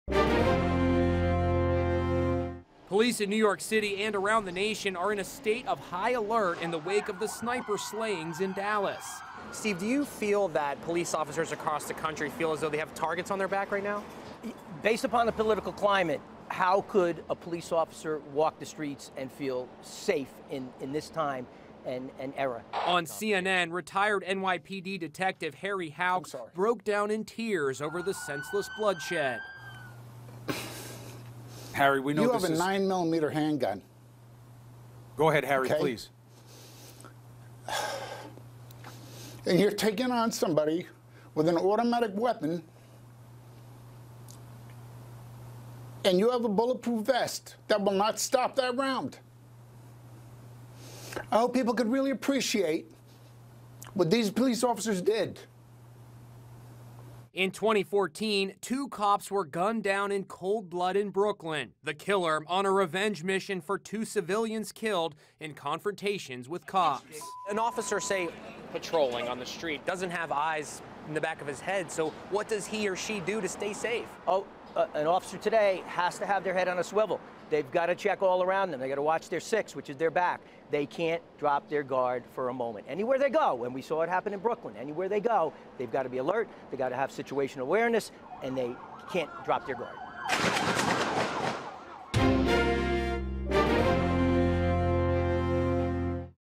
Police Officers In America Are On High Alert Following Dallas Sniper Attack
Police in New York City and around the national are in a state of high alert in the wake of the sniper attack in Dallas. Retired NYPD detective Harry Houck broke down in tears over the senseless bloodshed on CNN, "You have a 9mm hand gun and you're taking on somebody with an automatic weapon and you have a bullet proof vest that will not stop that round."